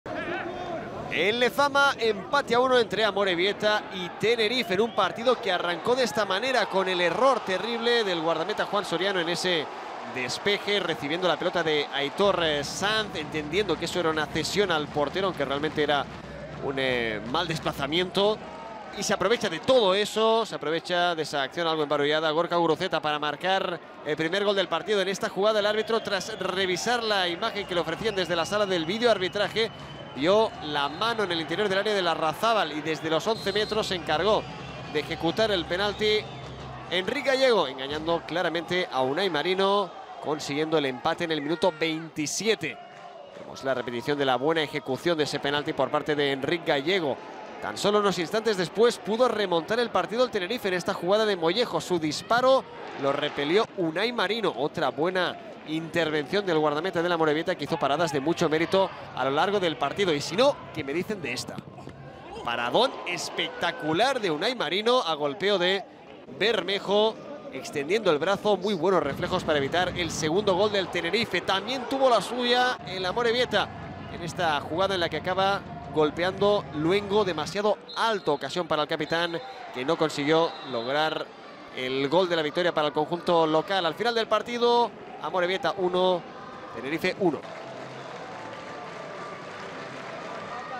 Resumen de SD Amorebieta vs CD Tenerife (1-1)
Empate a uno entre SD Amorebieta y CD Tenerife en Lezama. Guruzeta aprovechó un error de Juan Soriano para anotar el primer gol del partido y Enric Gallego igualó el marcador desde el punto de penalti AmorebietaTenerife J23 LaLiga SmartBank 2021/2022